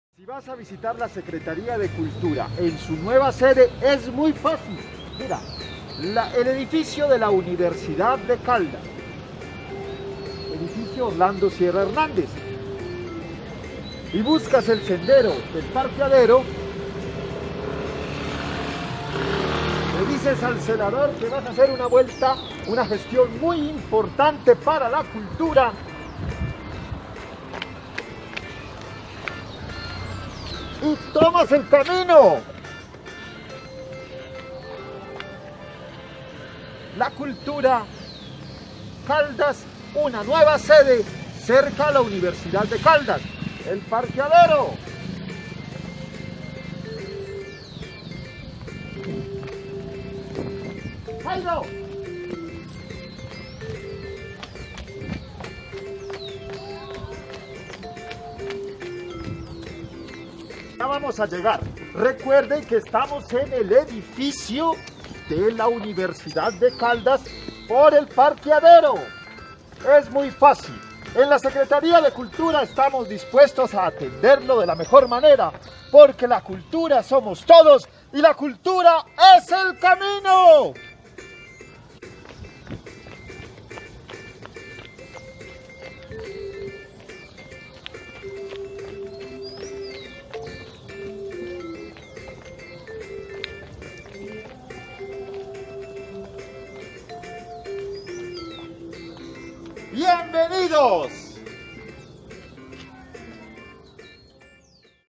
aquí te mostramos como llegar... a la nueva sede de la secretaria de cultura
Sabías que...en la sede de la secretaría de Cultura se están realizando obras de mantenimiento?. Por tal razón estaremos durante 4 meses en la universidad de Caldas, aquí te mostramos como llegar...